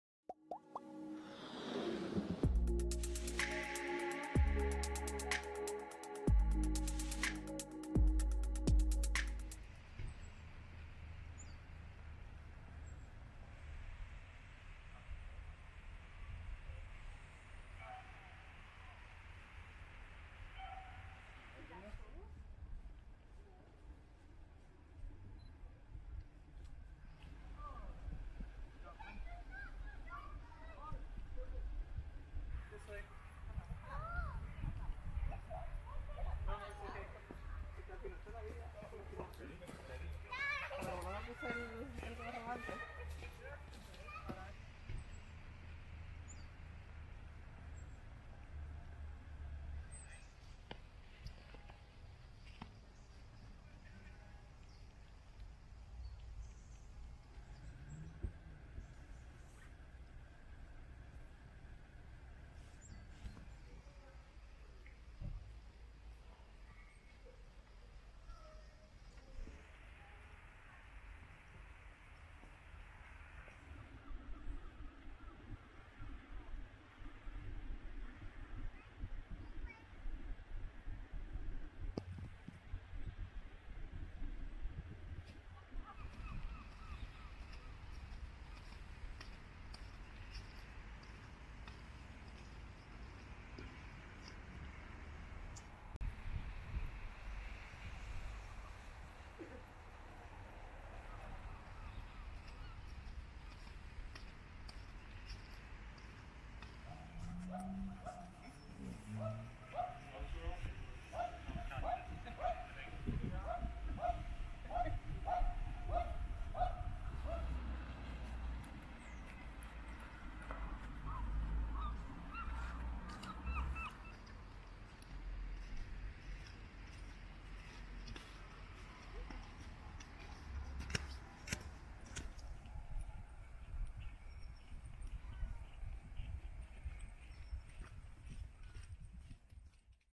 Snow in Bangor Northern Ireland (24th Jan 2021)
A rare snowfall in Bangor County Down in Northern Ireland from Bangor Town Hall and Castle Park, to the Seafront and Bangor Marina. Snow rarely lies in the coastal town of Bangor or Northern Ireland and the last time snow had lied it was 3 years earlier. This was a bit of our excitement this year during our year in lockdown (travel restrictions) stranding us in the U.K. All material is produced by travel bloggers Fanfan and Allan Wilson.